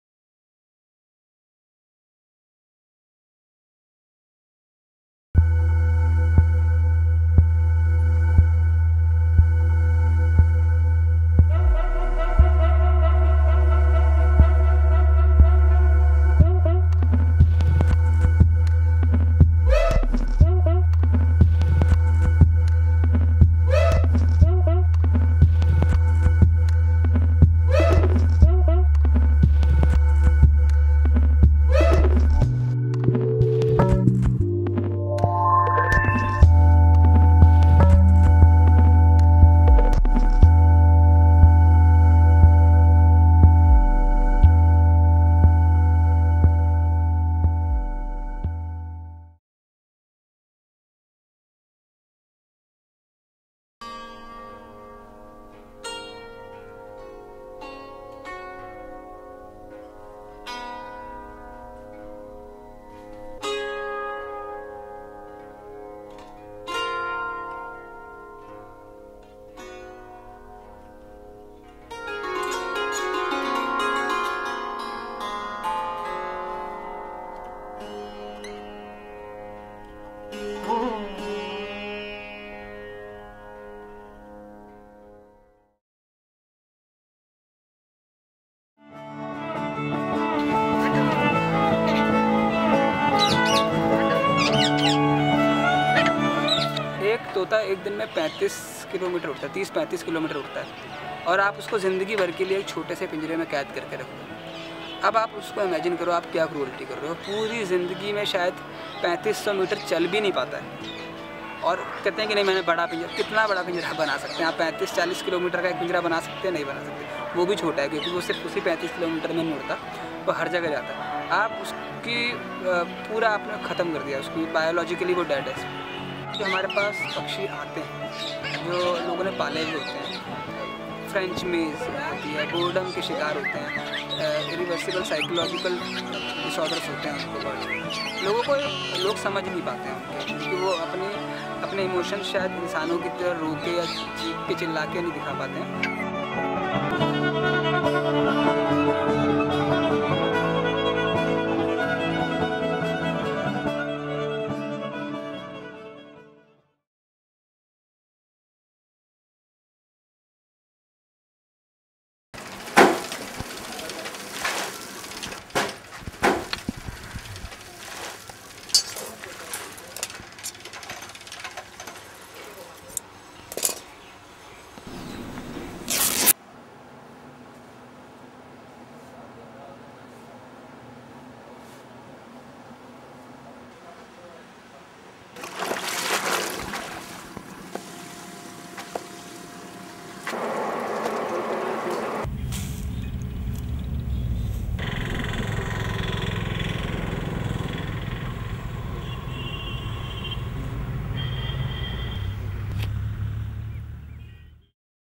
GrowlingPixels Documentary showreel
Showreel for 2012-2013

1) Painting with fire (Artist series)
2) Naba (Artist series)
3) Fauna Police
4) Kotadia (Artist series)

All work has been shot over weekends and with whatever resources available.

Shot on Canon 600D apart from Kotadia (Sony DVX100)